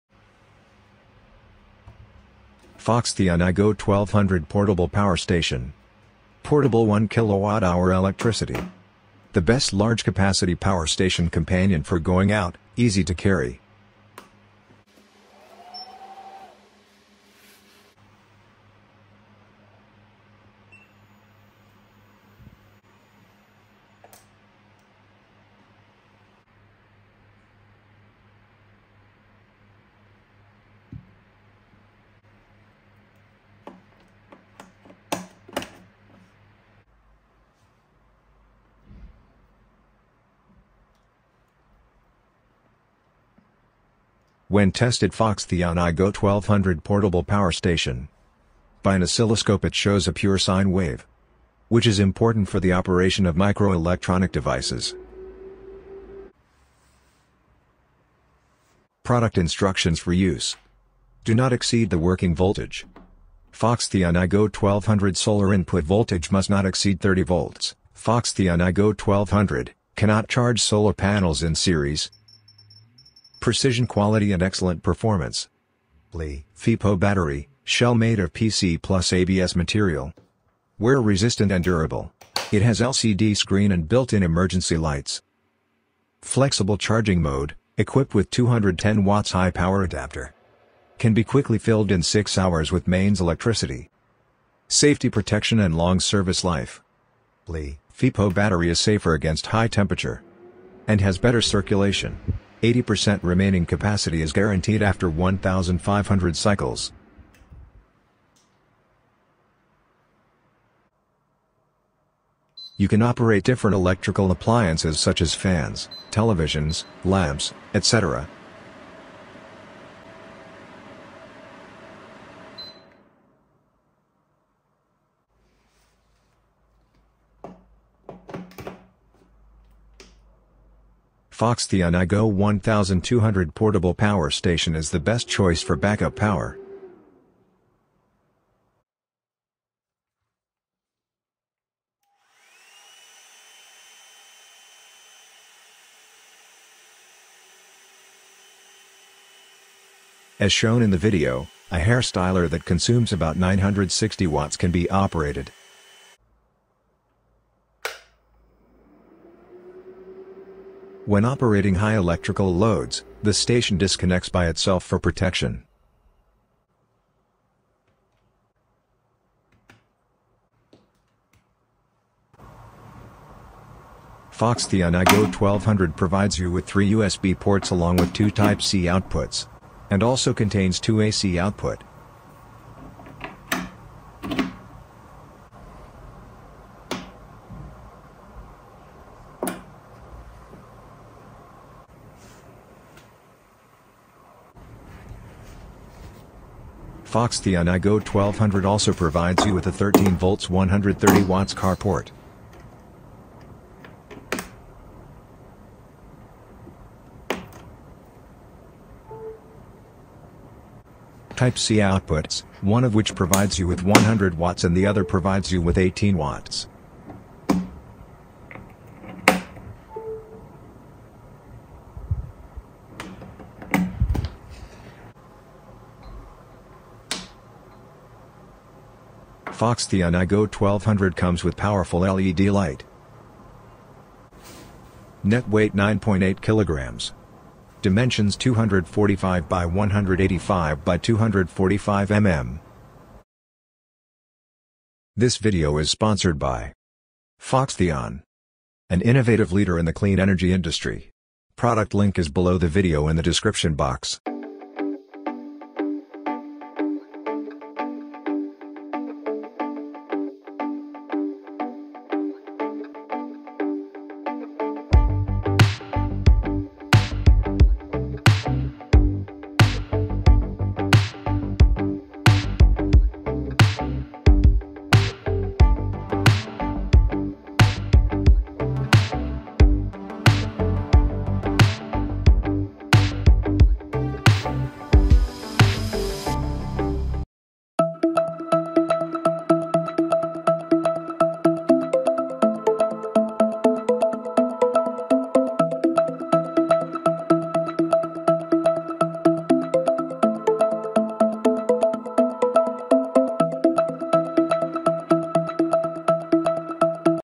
Foxtheon iGo1200 Power Station , Test and review
To buy all tools from here 👇👇
Unboxing and Review of the Foxtheon iGo1200 Power Station

And the rebate rate is 10% of the actural volume of transaction.

In this video, we will be unboxing and reviewing the Foxtheon iGo1200 Power Station, a versatile and portable power solution for outdoor enthusiasts, travelers, and emergency situations. This compact device is equipped with a powerful lithium-ion battery, capable of recharging smartphones, tablets, laptops, cameras, and other devices multiple times, thanks to its impressive 1200Wh capacity.

We will explore the features and specifications of the Foxtheon iGo1200 Power Station, including its multiple output options (AC, DC, USB), LCD display, built-in LED light, and sturdy design. We will also demonstrate how easy it is to recharge the device using solar panels or wall outlets, and how long it takes to fully charge.

Moreover, we will share our personal experience using the Foxtheon iGo1200 Power Station during camping trips, power outages, and other scenarios where reliable and efficient power is essential. We will highlight the pros and cons of this product, as well as compare it to other similar products in the market.

In conclusion, if you are looking for a dependable and versatile power station that can provide you with electricity on-the-go, the Foxtheon iGo1200 Power Station is definitely worth considering. Its excellent performance, user-friendly interface, and durable construction make it a great investment for anyone who values convenience and sustainability.

 Circuits diagrams and code can be found on the blog